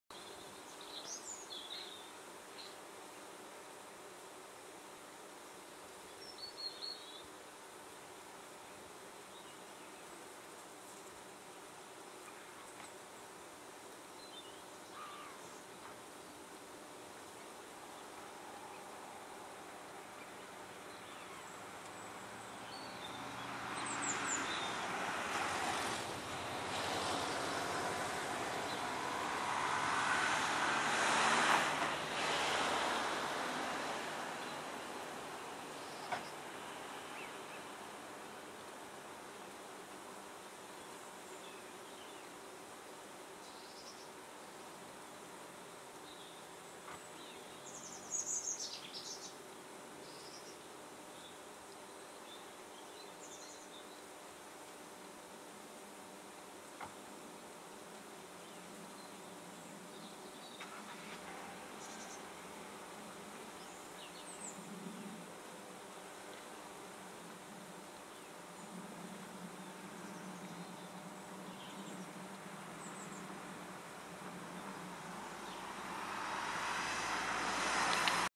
Painted Lady & friends
One of about 50 Painted Lady butterflies n an Ivy bush in Exmouth along with as many Red Admirals